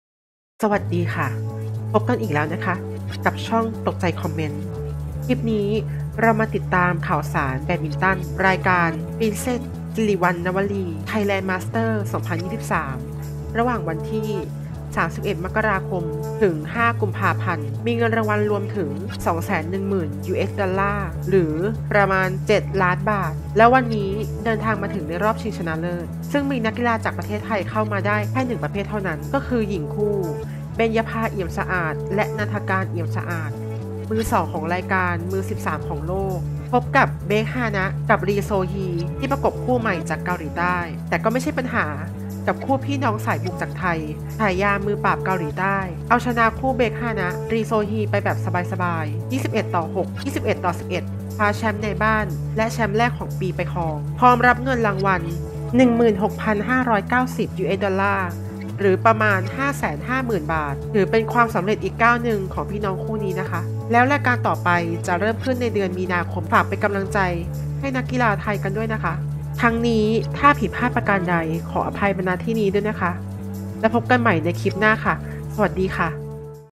สุดยอด "อันนา-มูนา" ซิวแชมป์หญิงคู่ แบดมินตัน "ไทยแลนด์ มาสเตอร์ส"
สรุปผลการแข่งขัน แบดมินตัน รายการ PRINCESS SIRIVANNAVARI Thailand Masters 2023 รอบ Final

เพื่อเป็นกำลังใจในการทำคลิปอีกทางด้วยนะคะ🙏💖